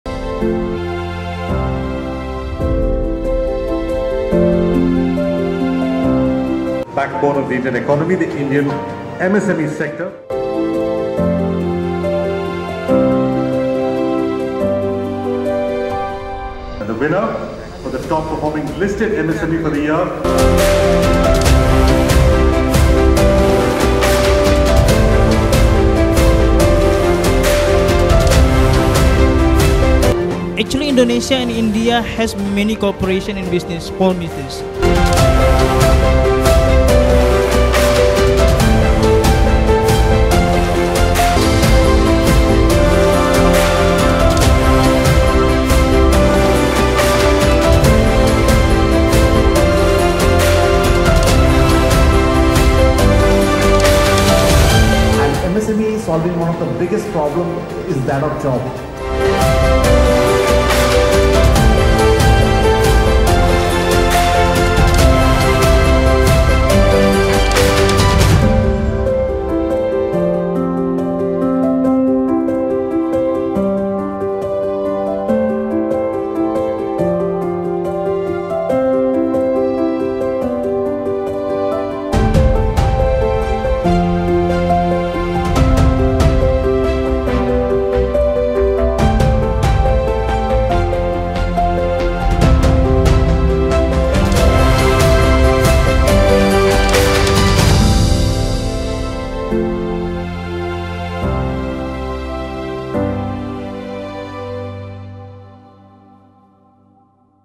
Economic Times - MSME Awards 2022 & 2023
Aver Media took pride in orchestrating the Economic Times MSME Awards 2022 & 2023, ensuring professionalism and integrity throughout. Our careful planning and execution made each ceremony memorable, highlighting the contributions of MSMEs. 💼📉